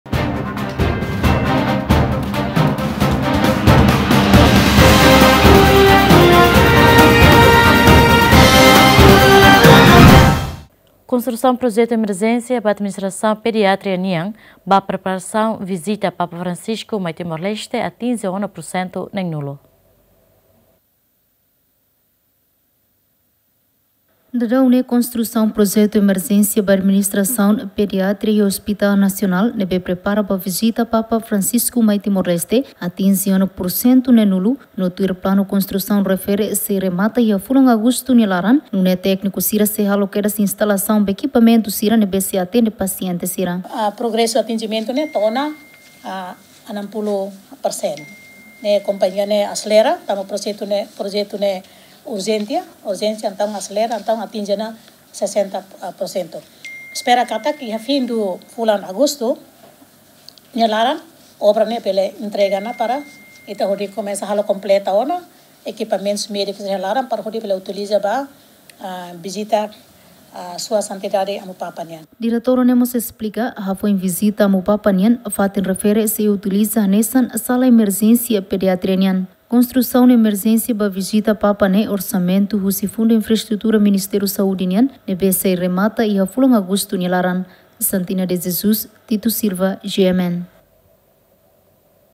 konstrusaun projetu emerjensia ba Administrasaun Pediatria nian ba preparasaun vizita Papa mai TL
konstrusaun projetu emerjensia ba Administrasaun Pediatria nian ba preparasaun vizita Papa mai Timor-Leste